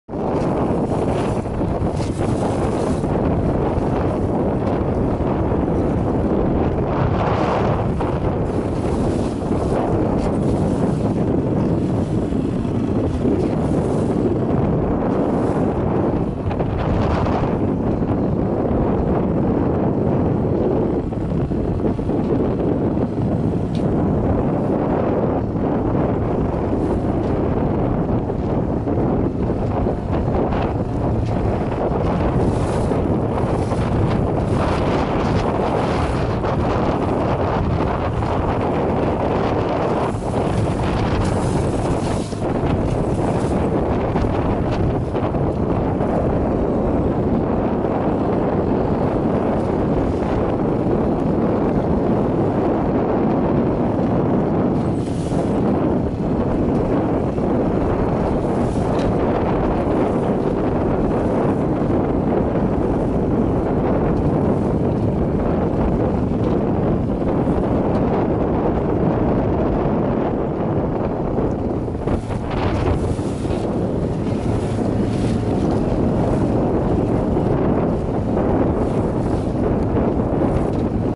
Against the chop!
Here's my S2 7.3 pounding against the chop on a windy day at the lake as we head for the racing starting line.